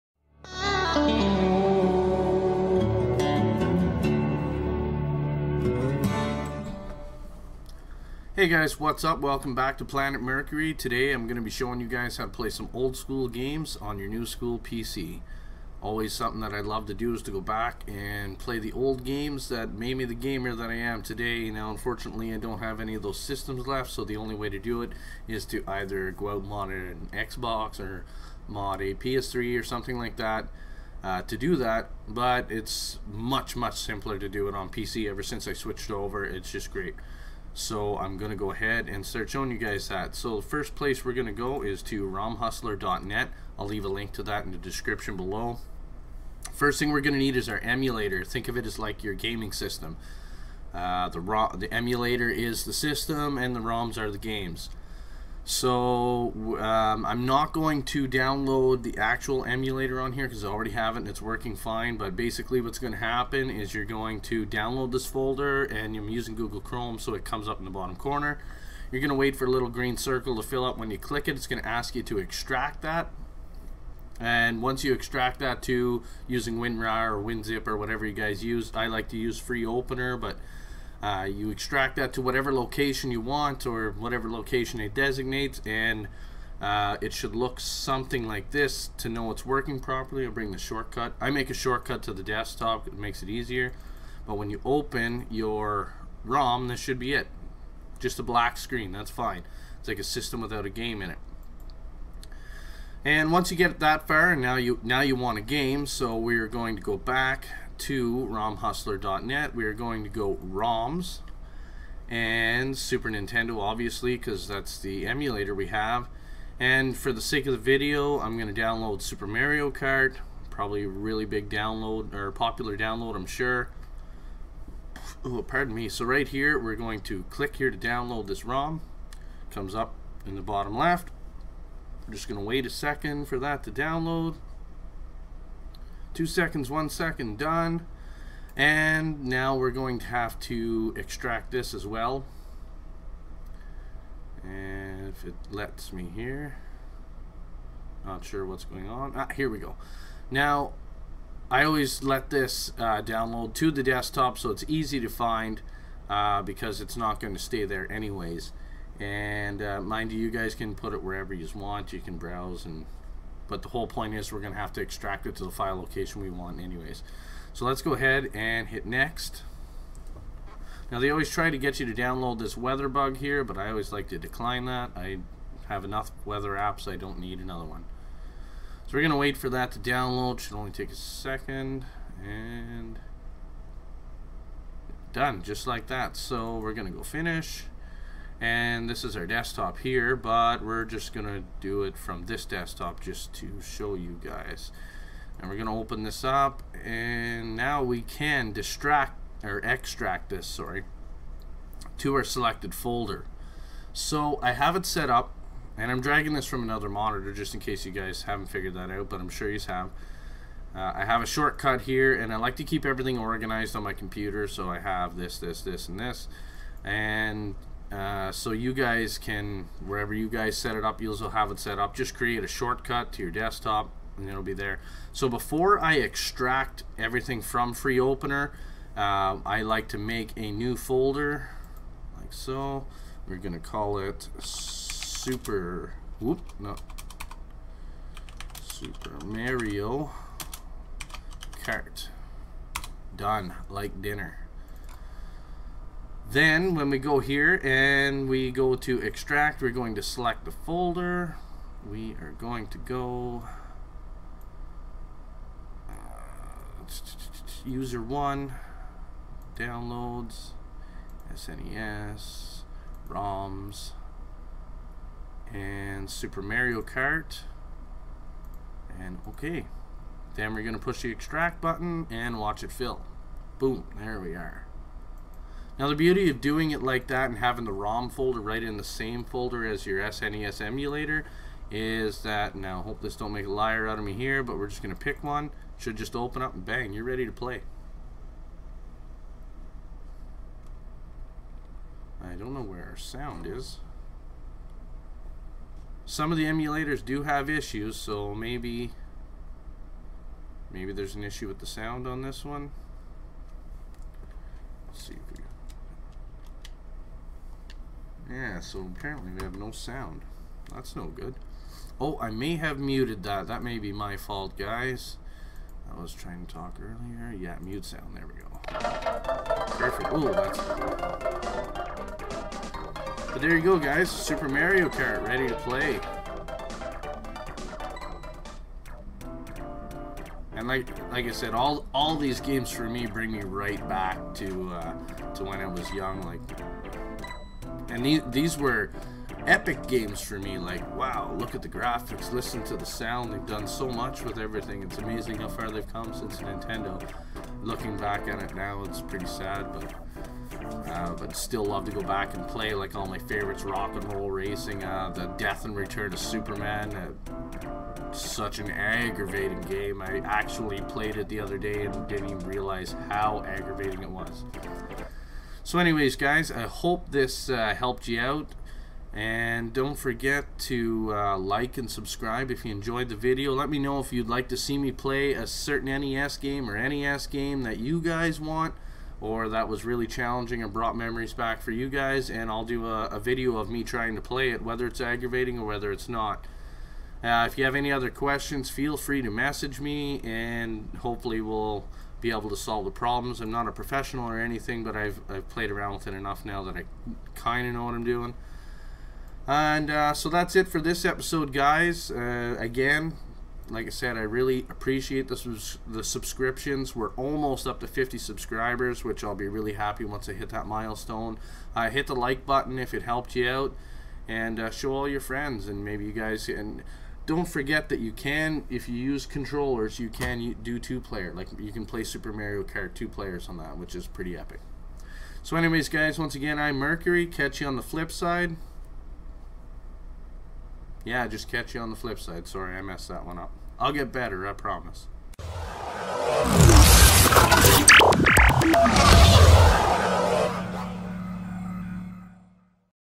How to play SNES on pc, Emulator
this is how you guys can play your old school snes games on your pc...hope you enjoy, and have fun...dont forget to like and subscribe, and also leave a comment..tell me your fav snes game